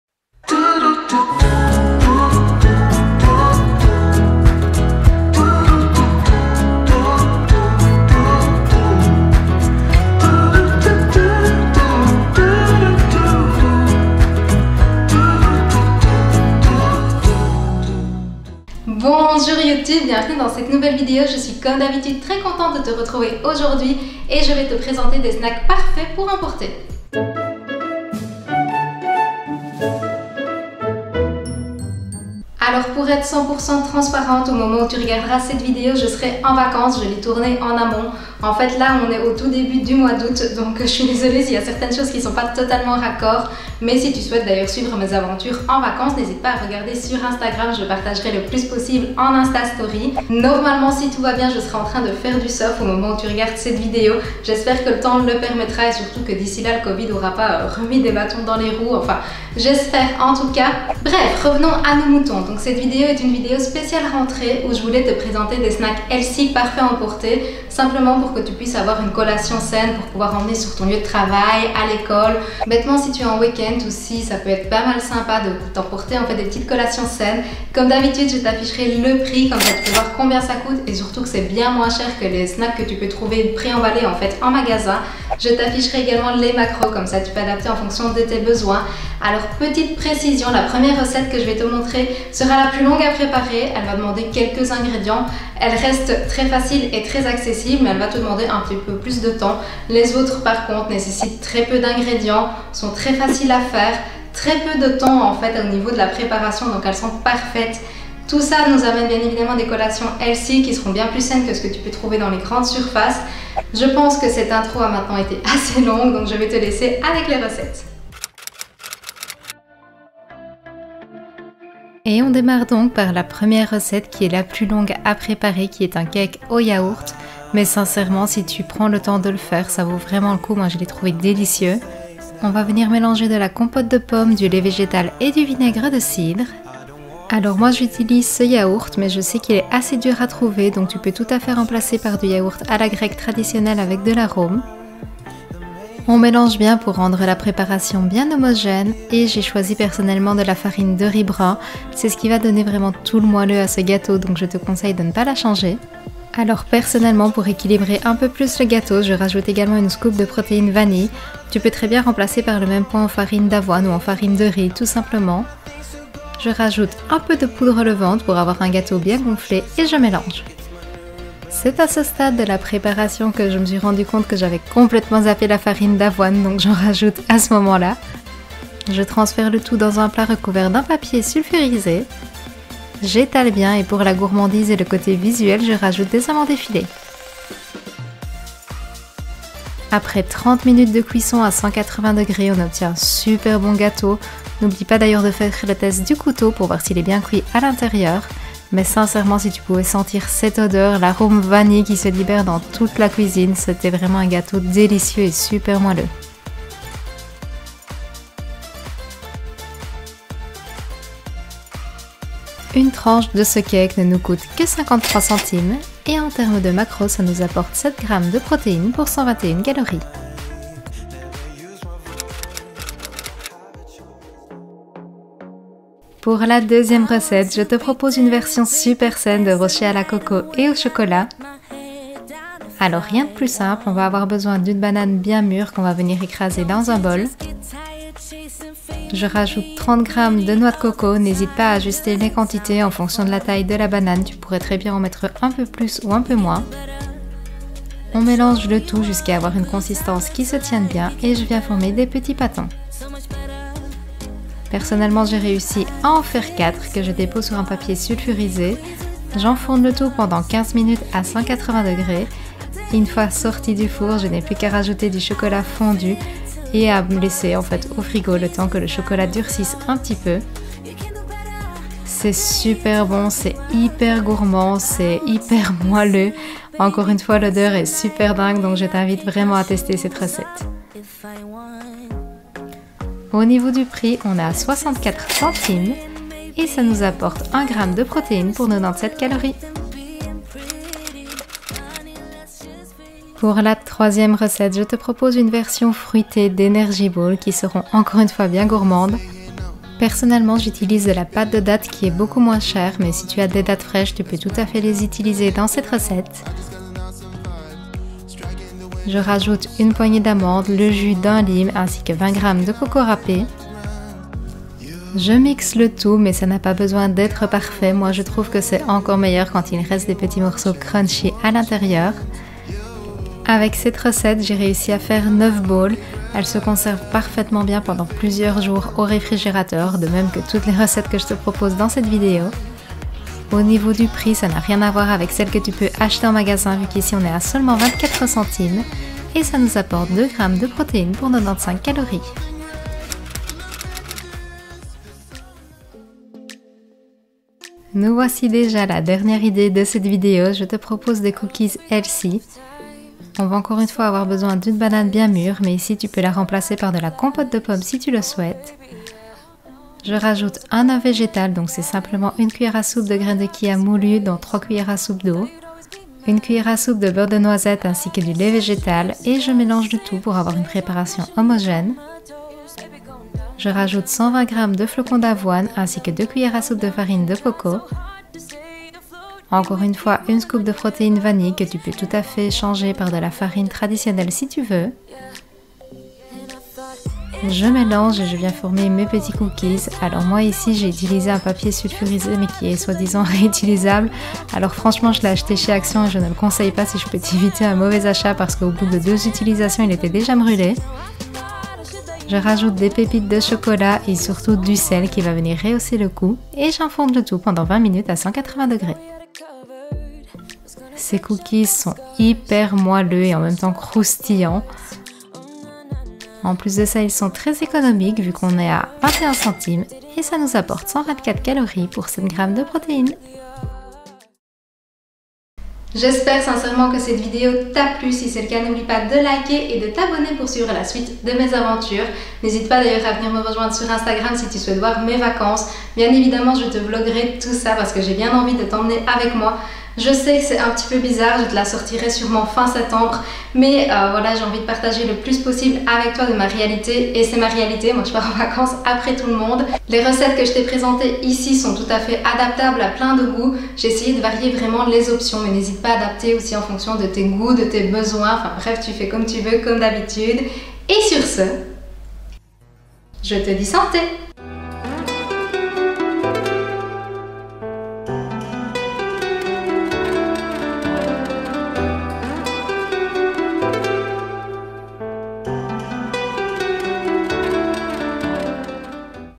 Back to work - Une semaine de Snacks healthy à emporter
Bonjour toi j'espère que tu vas bien ♡

Aujourd'hui je te propose des recettes très gourmande pour le retour au travail ou à l'école.
Il s'agit de snacks healthy facile à emporter qui se conservent très bien pendant plusieurs jours.
J'ai calculé  le prix de chaque recette pour que tu puisses avoir une idée budget ainsi que les macros.

𝓙'𝓮𝓼𝓹𝓮̀𝓻𝓮 𝓺𝓾𝓮 𝓬̧𝓪 𝓽𝓮 𝓹𝓵𝓪𝓲𝓻𝓪, 𝓮𝓽 𝓼𝓾𝓻 𝓬𝓮. 𝓙𝓮 𝓽𝓮 𝓭𝓲𝓼 𝓢𝓪𝓷𝓽𝓮́ !